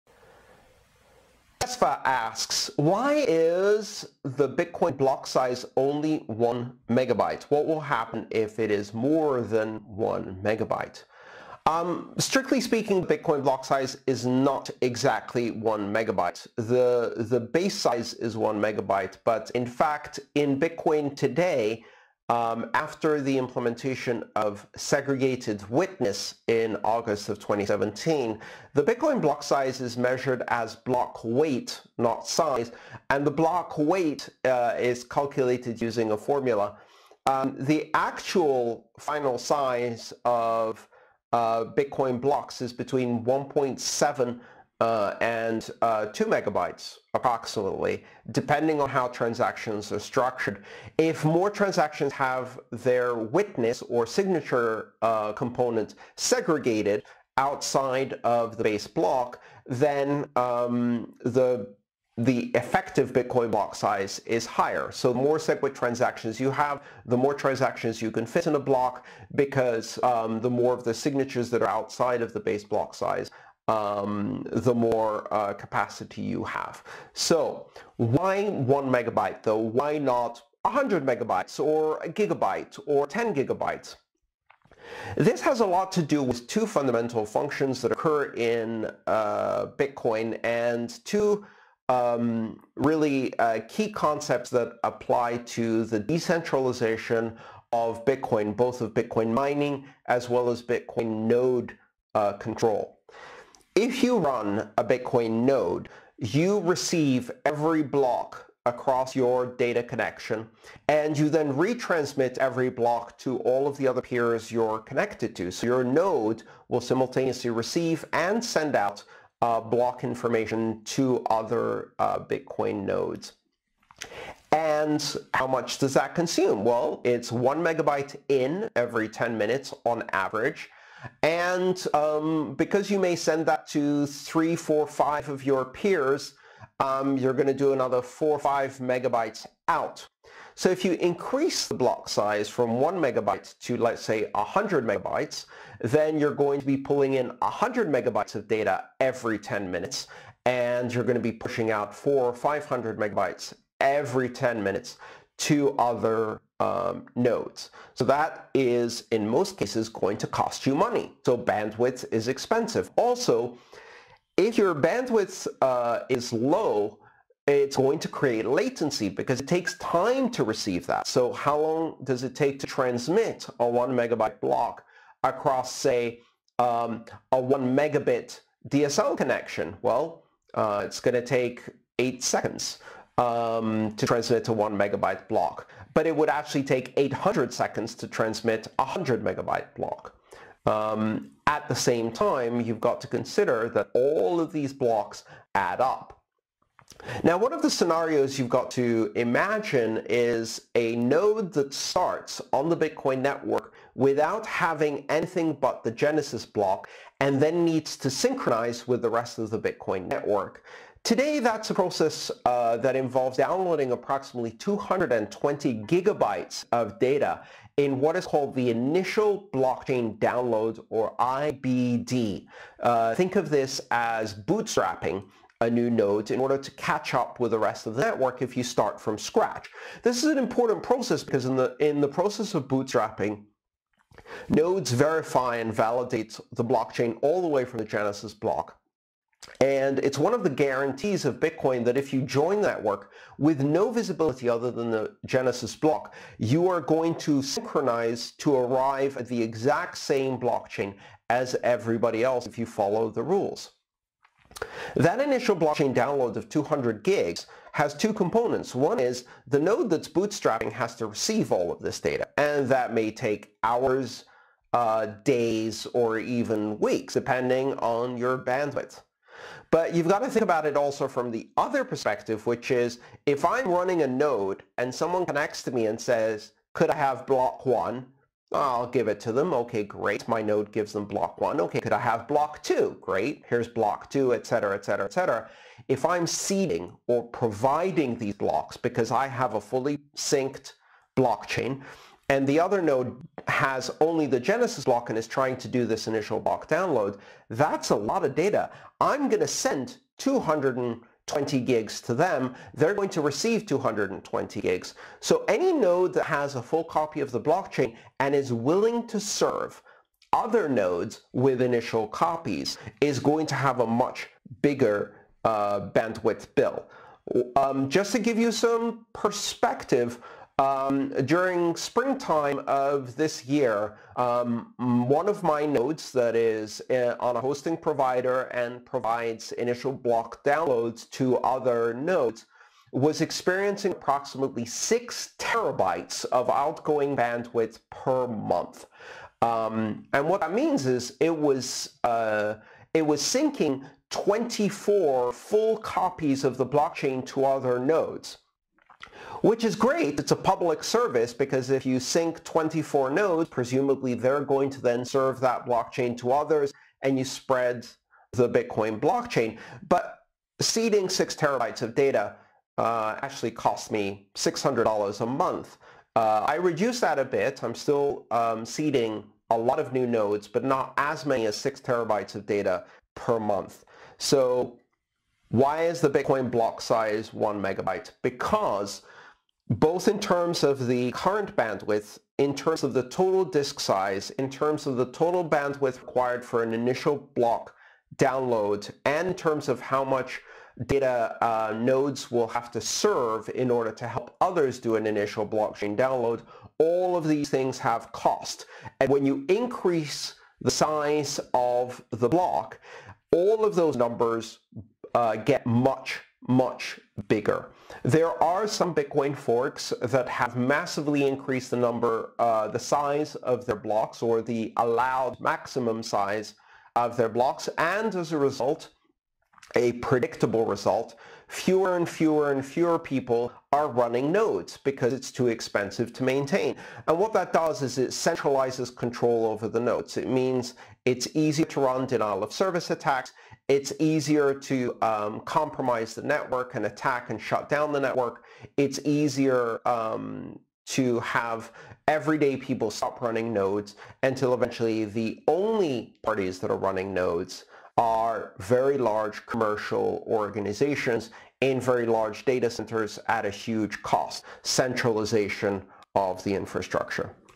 Why is Block Size 1MB
Short video from MOOC 12, Session 2: Introduction to Digital Currencies, with Andreas Antonopoulos, originally uploaded September 19th, 2019.